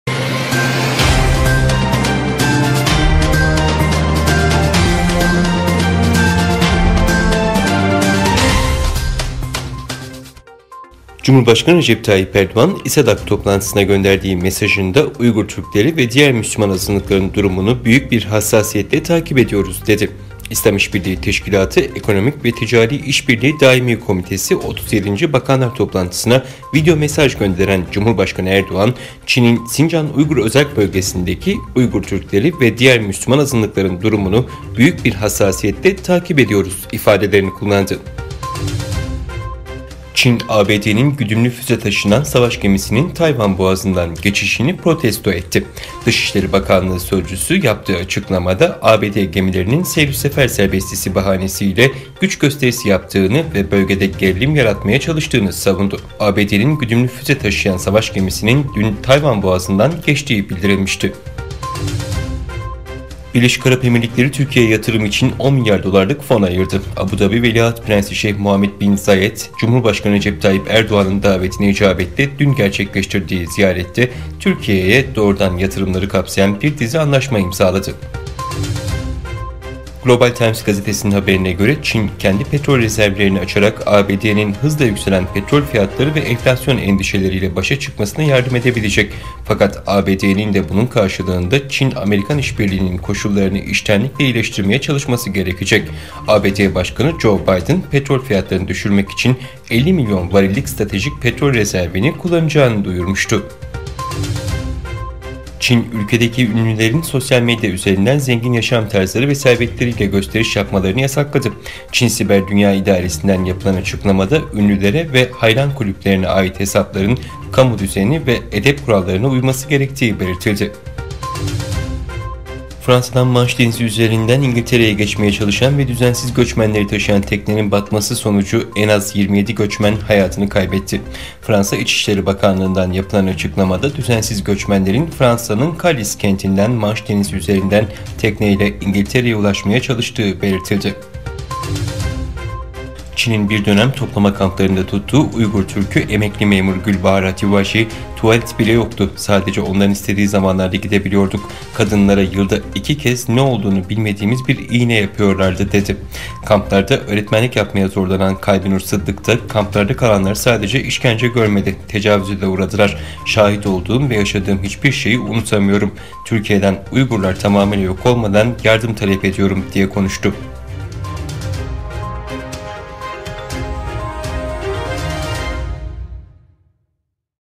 25/11/2021 BAE, Türkiye'ye yatırım için 10 milyar dolarlık fon ayırdı
Erdoğan: Uygur Türklerinin durumunu hassasiyetle takip ediyoruz
Cumhurbaşkanı Erdoğan, İSEDAK toplantısına gönderdiği mesajında, "Uygur Türkleri ve diğer Müslüman azınlıkların durumunu büyük bir hassasiyetle takip ediyoruz." dedi. Bakanlar Toplantısı'na video mesaj gönderen Cumhurbaşkanı Recep Tayyip Erdoğan, "Çin'in Sincan Uygur Özerk Bölgesi'ndeki Uygur Türkleri ve diğer Müslüman azınlıkların durumunu büyük bir hassasiyetle takip ediyoruz." dedi.

Çin, ABD savaş gemisinin Tayvan Boğazı’ndan geçmesini protesto etti
Çin, ABD'nin güdümlü füze taşınan savaş gemisinin Tayvan Boğazı'ndan geçişini protesto etti. Dışişleri Bakanlığı Sözcüsü Cao Licien, yaptığı açıklamada, ABD gemilerinin seyrüsefer serbestisi bahanesiyle güç gösterisi yaptığını ve bölgede gerilim yaratmaya çalıştığını savundu. ABD'nin güdümlü füze taşınan "USS Milius" savaş gemisinin dün Tayvan Boğazı'ndan geçtiği bildirilmişti.

BAE, Türkiye'ye yatırım için 10 milyar dolarlık fon ayırdı
Birleşik Arap Emirlikleri, Türkiye'ye yatırım için 10 milyar dolarlık fon ayırdı. Abu Dabi Veliaht Prensi Şeyh Muhammed bin Zayed Al Nahyan'ın, Cumhurbaşkanı Recep Tayyip Erdoğan'ın davetine icabetle dün gerçekleştirdiği ziyarette, Türkiye'ye doğrudan yatırımları kapsayan bir dizi anlaşma imzalandı.

Global Times gazetesinin haberine göre Çin, kendi petrol rezervlerini açarak ABD'nin hızla yükselen petrol fiyatları ve enflasyon endişeleriyle başa çıkmasına yardım edebilecek, fakat ABD’nin de bunun karşılığında Çin-Amerikan işbirliğinin koşullarını içtenlikle iyileştirmeye çalışması gerekecek. ABD Başkanı Joe Biden, petrol fiyatlarını düşürmek için 50 milyon varillik stratejik petrol rezervini kullanacağını duyurmuştu.

Çin, ünlülerin sosyal medyada gösteriş yapmasını yasakladı
Çin Siber Dünya İdaresi'nden yapılan açıklamada ünlülere ve hayran kulüplerine ait hesapların kamu düzeni ve edep kurallarına uyması gerektiği belirtildi.

Manş Denizi'nde bir teknenin batması sonucu en az 27 düzensiz göçmen öldü
Fransa'dan Manş Denizi üzerinden İngiltere'ye geçmeye çalışan ve düzensiz göçmenleri taşıyan teknenin batması sonucu en az 27 göçmen hayatını kaybetti. Fransa İçişleri Bakanlığından yapılan açıklamada, düzensiz göçmenlerin Fransa'nın Calais kentinden Manş Denizi üzerinden tekneyle İngiltere'ye ulaşmaya çalıştığı belirtildi.

“Uygurlar yok edilmeden Türkiye el uzatsın”
Çin'in bir dönem toplama kamplarında tuttuğu Uygur Türkü emekli memur Gülbahar Hatiwaji "Tuvalete bile sadece onların istediği zamanlarda gidebiliyorduk. Kadınlara yılda iki kez ne olduğunu bilmediğimiz bir iğne yapıyorlardı" dedi. Kamplarda öğretmenlik yapmaya zorlanan Kalbinur Sıddık da "Kamplarda kalanlar sadece işkence görmedi, tecavüze de uğradılar. Şahit olduğum ve yaşadığım hiçbir şeyi unutamıyorum. Türkiye'den, Uygurlar tamamıyla yok olmadan yardım talep ediyorum" diye konuştu.